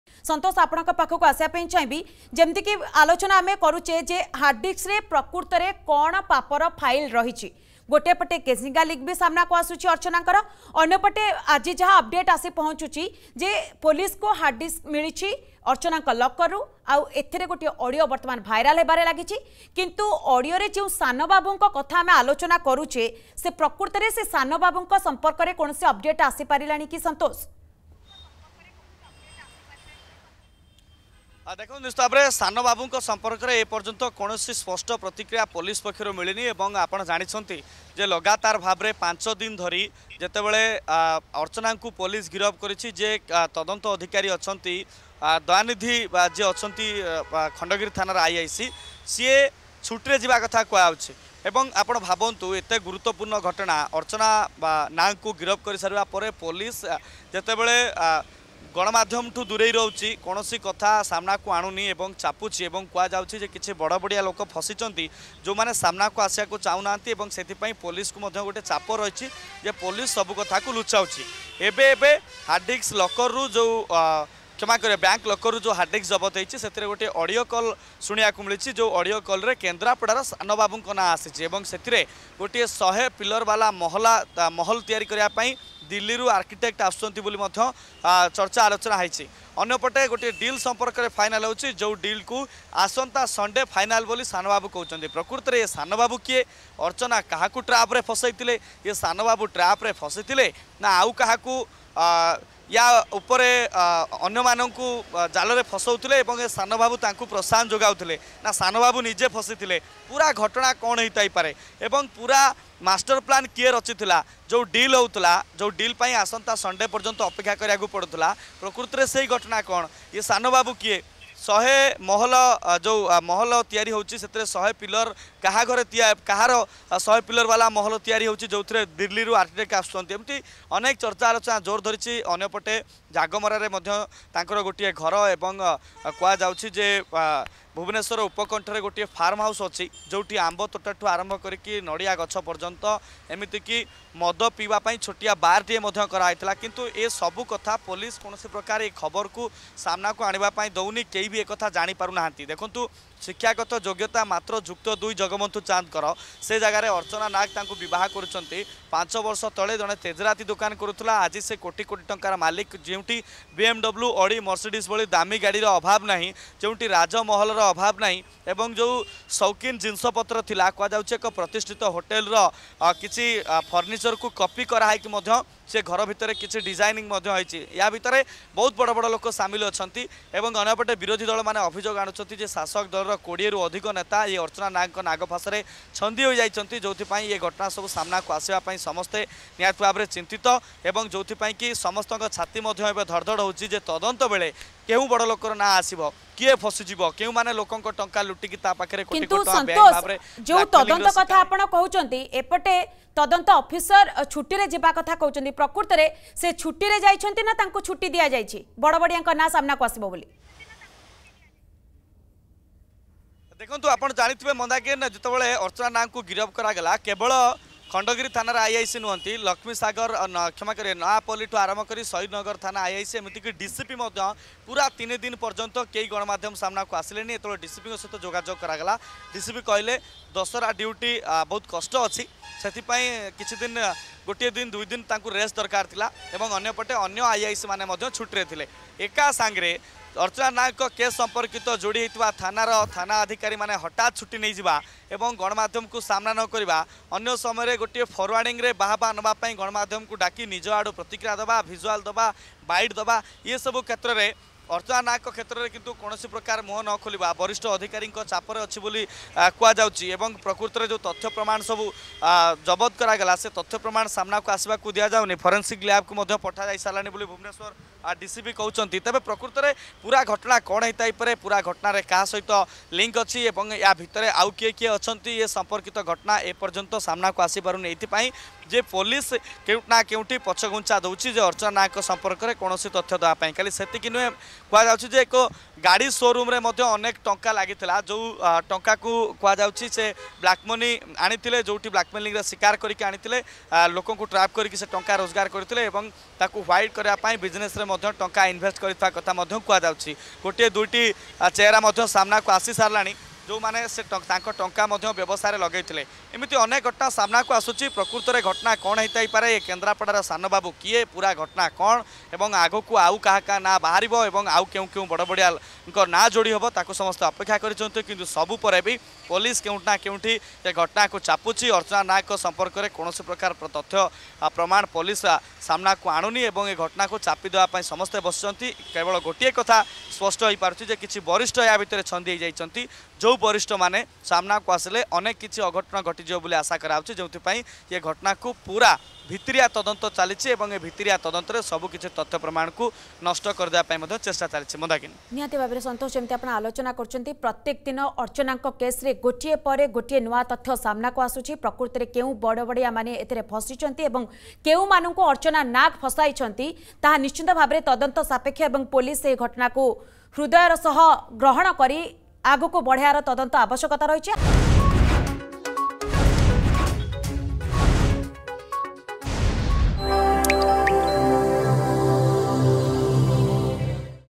ହାର୍ଡ ଡିସ୍କ୍‌ରେ କେଉଁ ପାପର ଫାଇଲ୍‌ ଅଛି ?
ମାୟାବିନୀ ଅର୍ଚ୍ଚନାଙ୍କ ଜାଲରେ କିଏ ସେ ସାନ ବାବୁ ? ଏ ପର୍ଯ୍ୟନ୍ତ ସେ ସମ୍ପର୍କରେ କିଛି ସ୍ପଷ୍ଟ ସୂଚନା ମିଳିପାରିନାହିଁ. ।